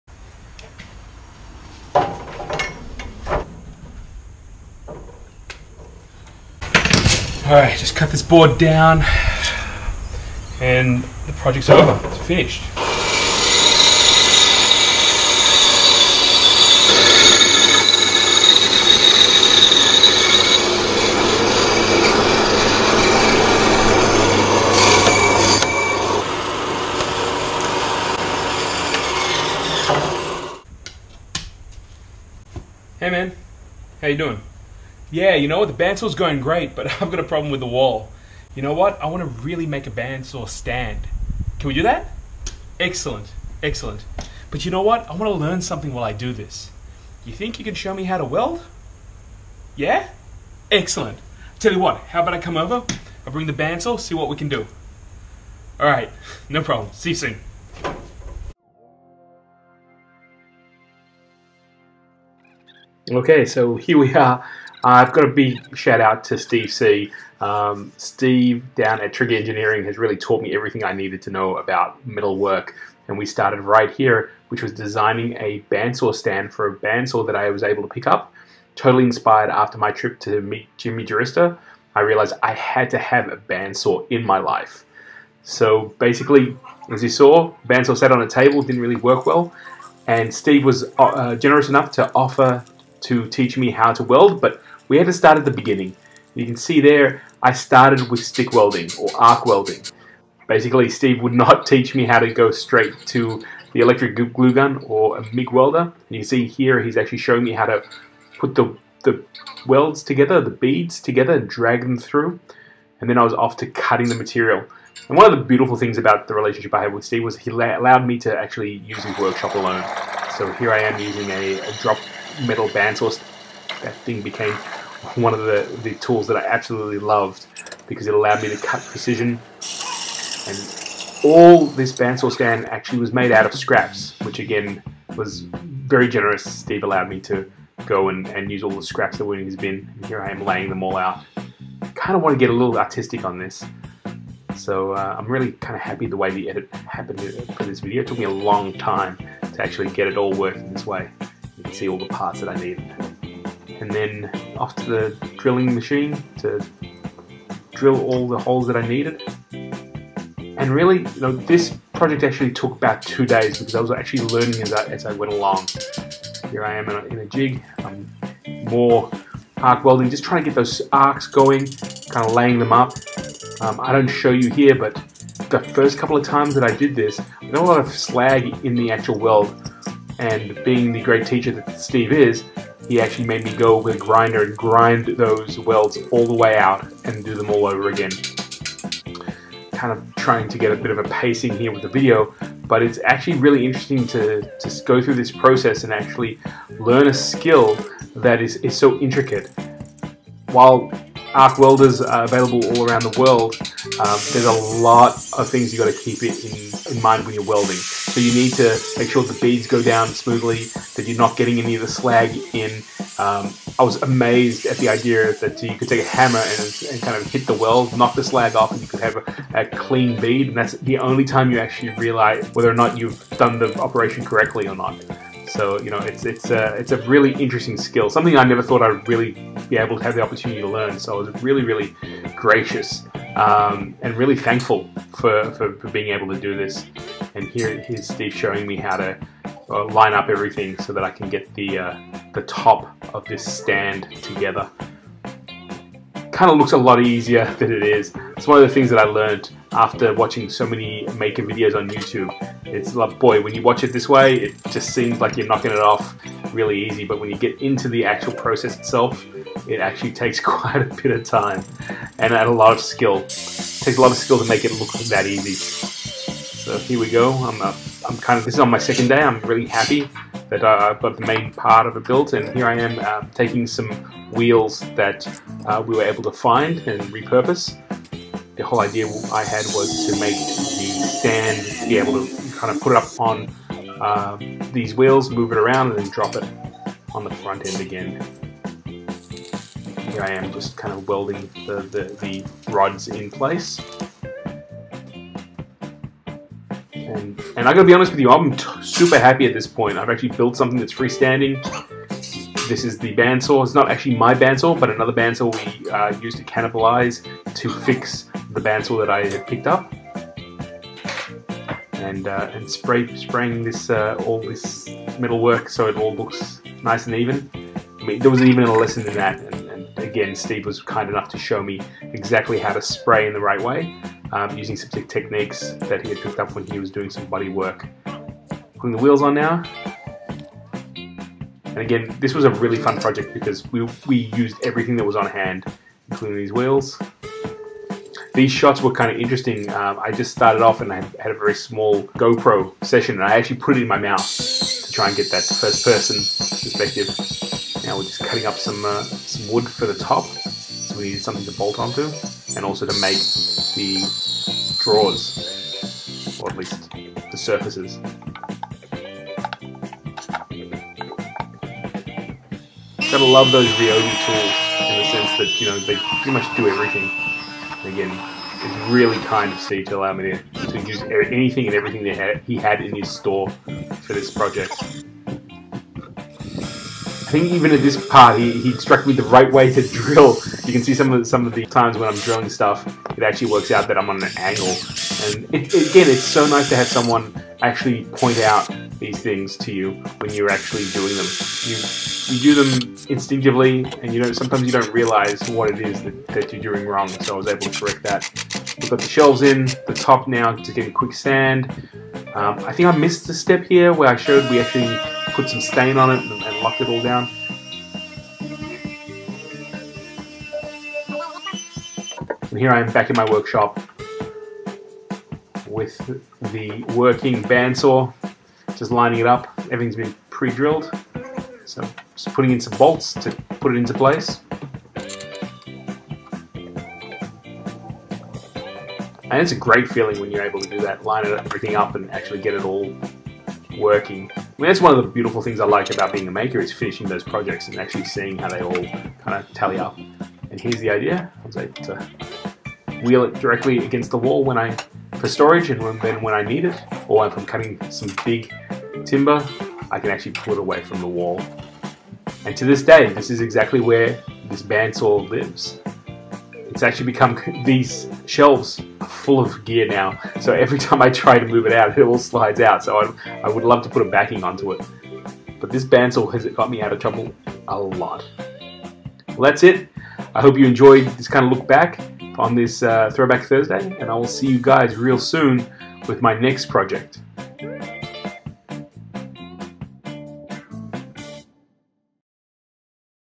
How to Arc weld learn the basics like a boss
Learn to arc weld like a boss - beginners journey.

Welding is the cornerstone of metalworking and it's also an essential skill needed for fabricating.
The first steps are to learn how to arc weld, which allows you to build up your skills before going on with MIG or TIG welding - but these basic skills are imperative so they can be learned early!

The original post was written back in April 2016; this updated version has new voiceovers explaining what we're doing as well as some extra tidbits from me along my journey while learning everything there is about sticking joints that will make any fabrication project complete (and safe!)

After fixing my bandsaw, I headed down to Steve Cs to weld up a stand for my new bandsaw and learn some new skills along the way.